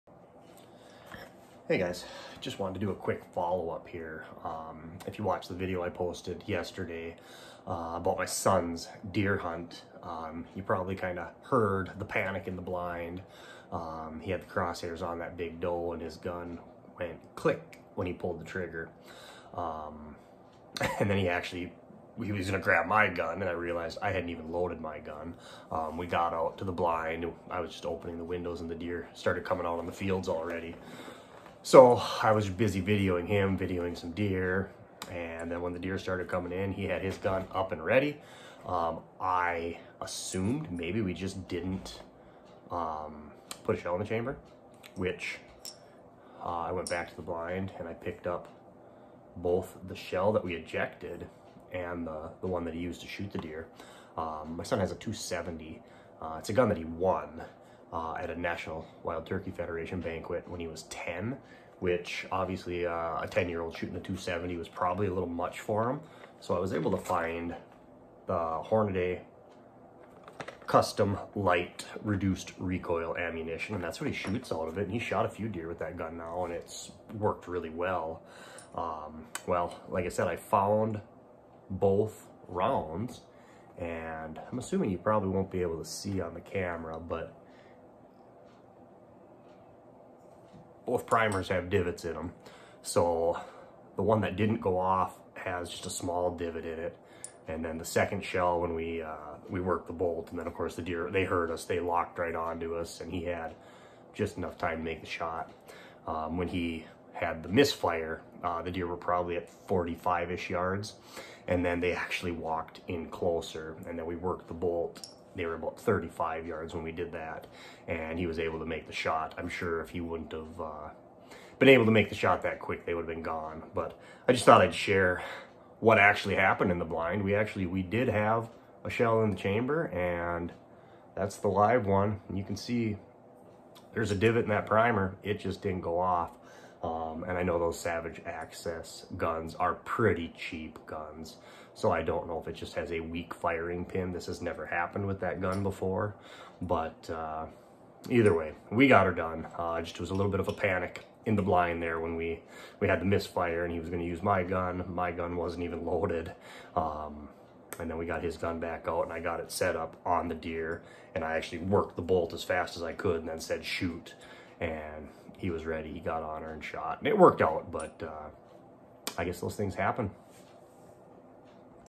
Miss-fire from the Deerstand.￼
We had a mis-fire from our Savage 270!